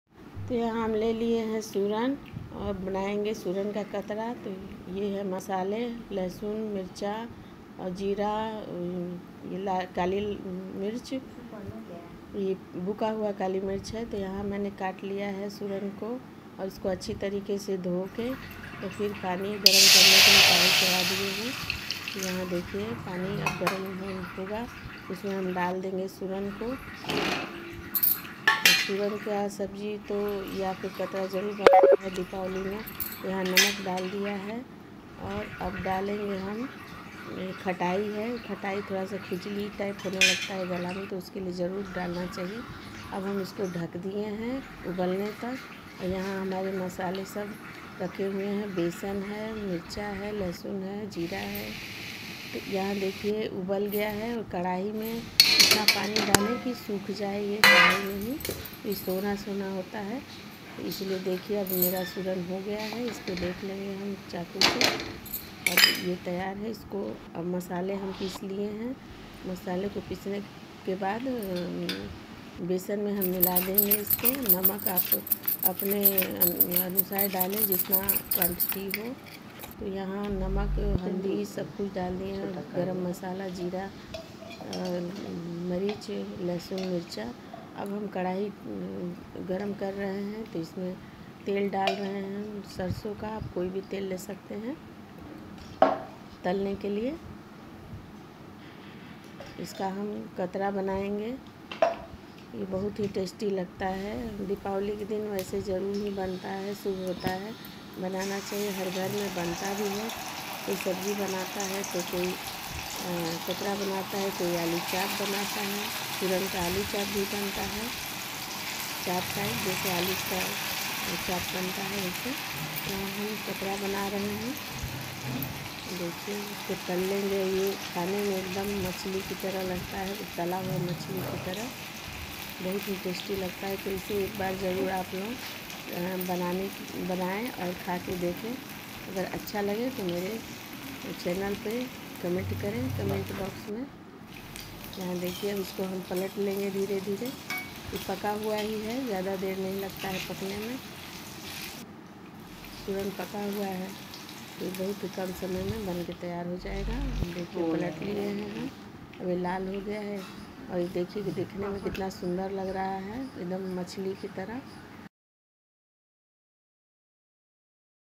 Diwali Special| Suran ka katra
1/2 kg Suran
Besan
Masale: Jeera
               Black Pepper
              Aamchur
             Salt
            Turmeric
           Garlic & Green chilli paste